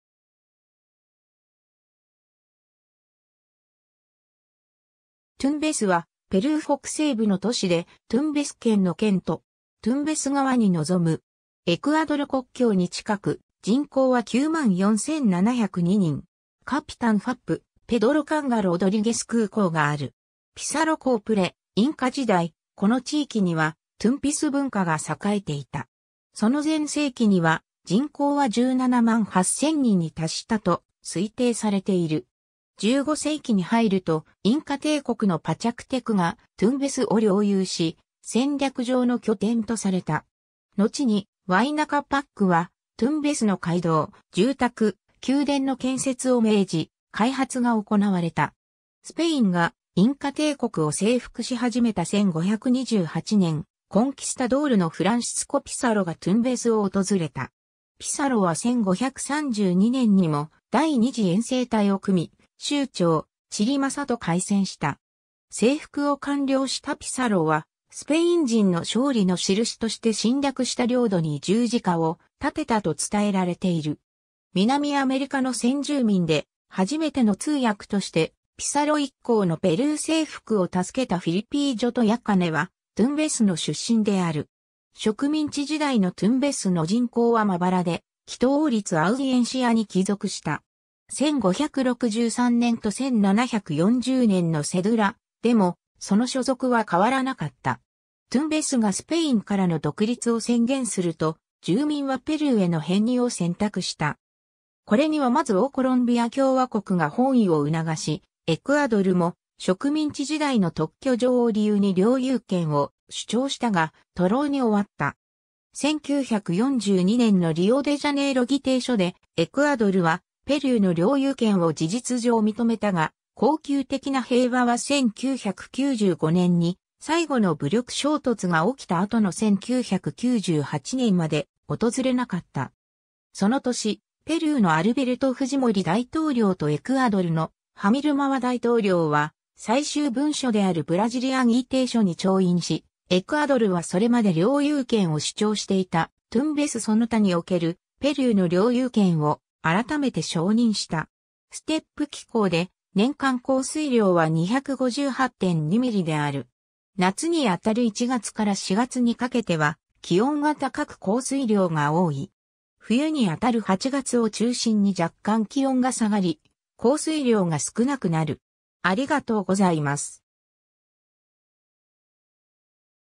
トゥンベス
トゥンベス（スペイン語: Tumbes）は、ペルー北西部の都市で、トゥンベス県の県都。
トゥンベス川に臨む。
エクアドル国境に近く、人口は9万4702人。
カピタンFAPペドロ・カンガ・ロドリゲス空港がある。
 ピサロ港 プレ・インカ時代、この地域にはトゥンピス文化が栄えていた。
その全盛期には、人口は17万8000人に達したと推定されている。
15世紀に入ると、インカ帝国のパチャクテクがトゥンベスを領有し、戦略上の拠点とされた。
のちにワイナ・カパックはトゥンベスの街道、住宅、宮殿の建設を命じ、開発が行われた。
 スペインがインカ帝国を征服しはじめた1528年、コンキスタドールのフランシスコ・ピサロがトゥンベスを訪れた。
ピサロは1532年にも第二次遠征隊を組み、酋長チリマサと会戦した（マングラーレスの戦い）。
征服を完了したピサロは、スペイン人の勝利の印として侵略した領土に十字架をたてたと伝えられている。
南アメリカの先住民で初めての通訳として、ピサロ一行のペルー征服を助けたフィリピージョとヤカネは、トゥンベスの出身である。
 植民地時代のトゥンベスの人口はまばらで、キト王立アウディエンシアに帰属した。
1563年と1740年のセドゥラ（特許状）でも、その所属は変わらなかった。
トゥンベスがスペインからの独立を宣言すると、住民はペルーへの編入を選択した。
これにはまず大コロンビア共和国が翻意をうながし、エクアドルも植民地時代の特許状を理由に領有権を主張したが、徒労に終わった。
1942年のリオデジャネイロ議定書で、エクアドルはペルーの領有権を事実上認めたが、恒久的な平和は1995年に最後の武力衝突が起きたあとの1998年まで訪れなかった。
その年、ペルーのアルベルト・フジモリ大統領とエクアドルのハミル・マワ大統領は最終文書であるブラジリア議定書に調印し、エクアドルはそれまで領有権を主張していた、トゥンベスその他におけるペルーの領有権を改めて承認した。
 ステップ気候で、年間降水量は258.2ミリである。
夏に当たる1月から4月にかけては気温が高く降水量が多い。
冬に当たる8月を中心に若干気温が下がり、降水量が少なくなる。